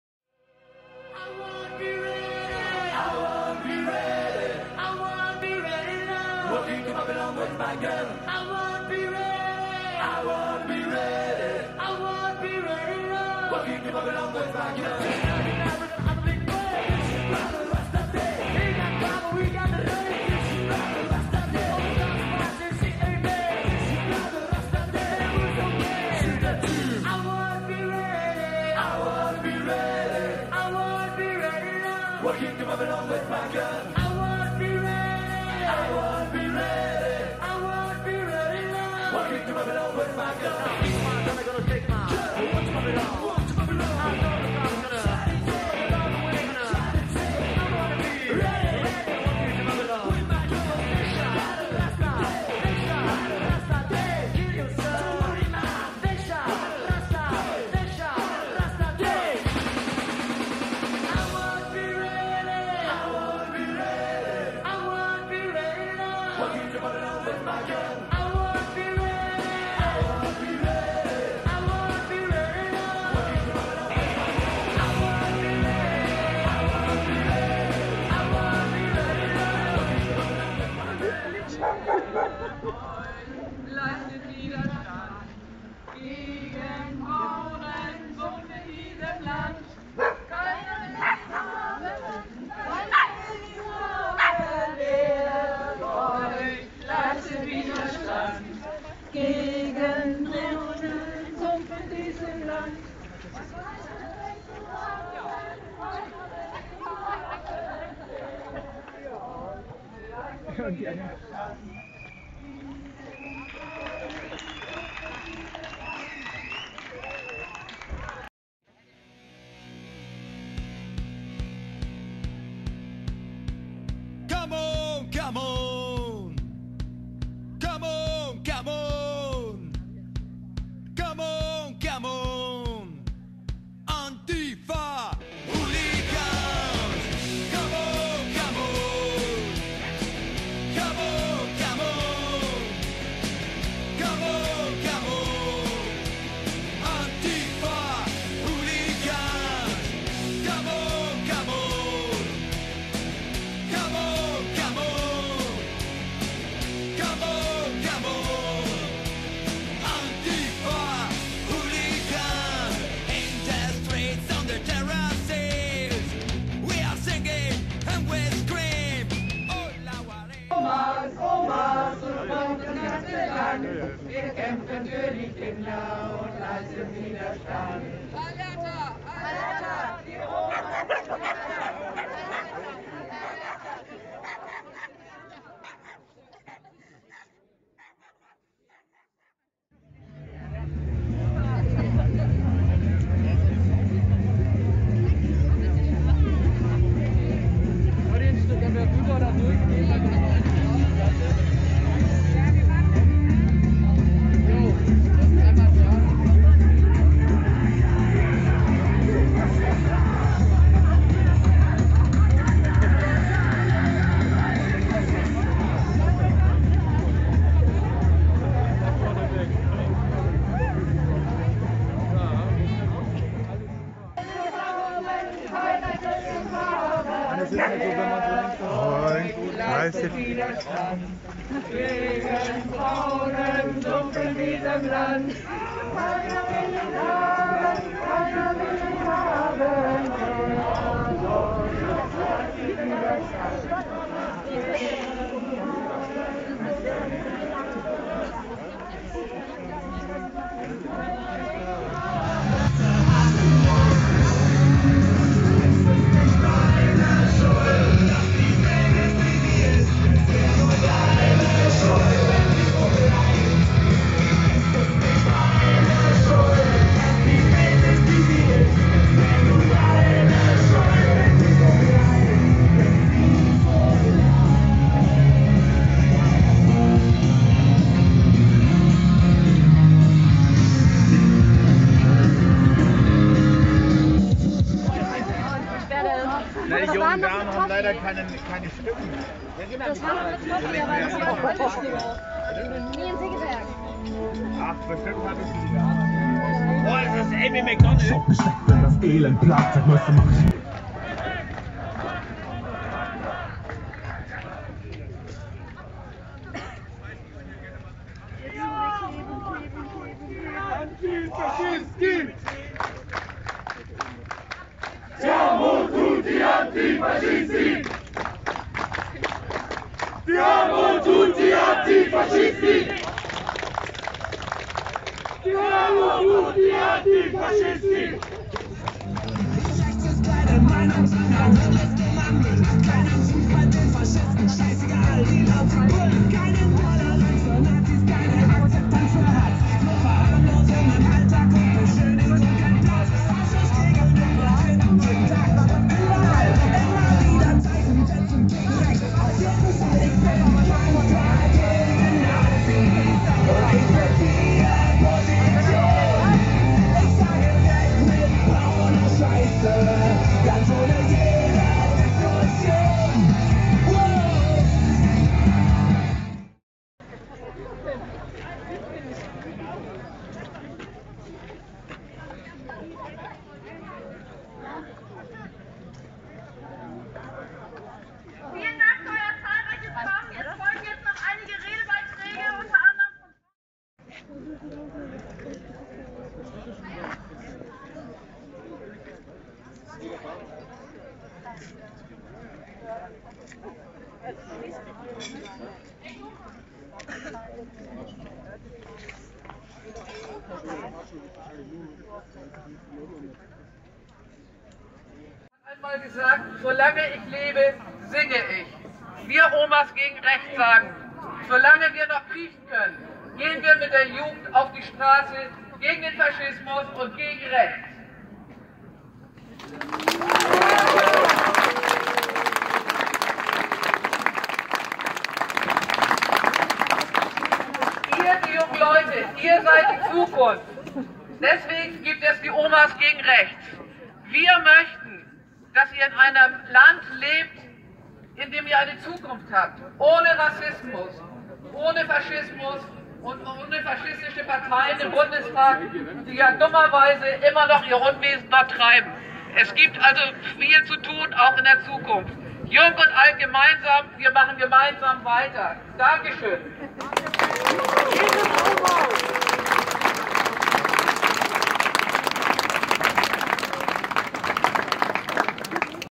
Das Herzogtum bleibt Nazifrei. 10.10.2021 Demo gegen Rechts mit den Omas gegen Rechts.
Demo in Schwarzenbek und die Omas gegen Rechts vorn mit dabei.